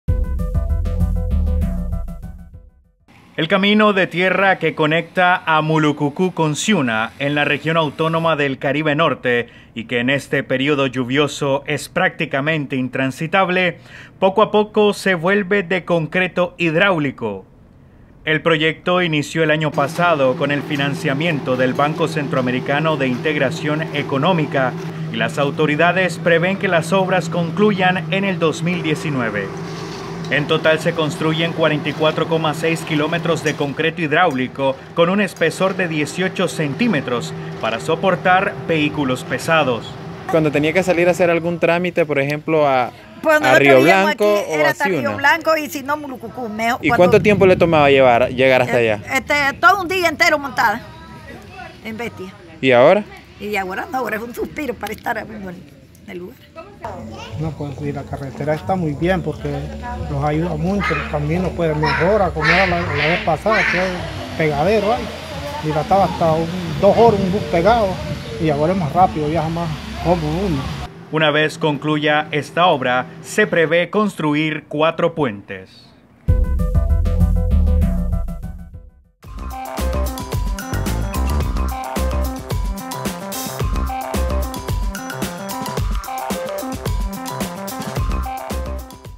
Avanza construcción de carretera Mulukukú Siuna
avances • construccion de carretera • mulukuku • siuna • region autonoma • caribe norte • nicaragua • tv • video • noticias • noticiero • canal • medios

Se construyen 44.6 kilómetros de concreto hidráulico que conectan a Mulukukú con Siuna en la Región Autónoma del Caribe Norte.